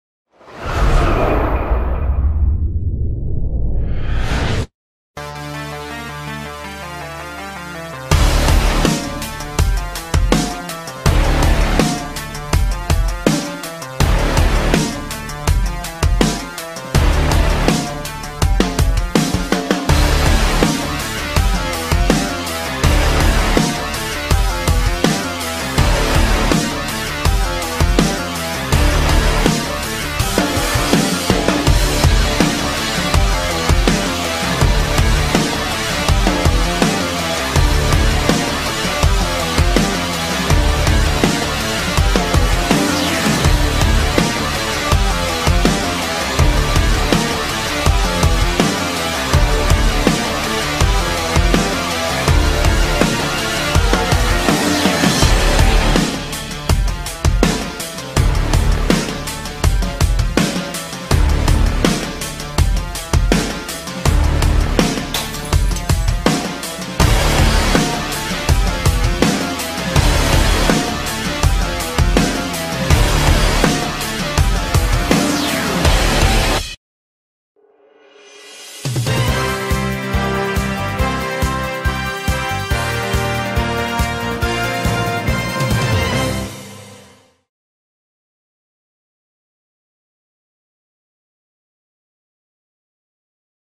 «Ростов» - «Локомотив» - 0:1. Обзор матча
Первый гол Эдера за «Локомотив» принес нашей команде победу над «Ростовом» в 11-м туре РОСГОССТРАХ Чемпионата России. Обзор матча от «Локо-ТВ».